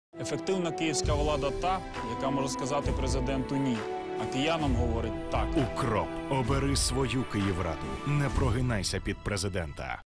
Команда УКРОПу йде в Київраду №7
Рекламний ролік кандидатів у депатути Київради від партії “УКРОП”

Замовник: Політична Партія “Українське об’єднання патріотів – УКРОП”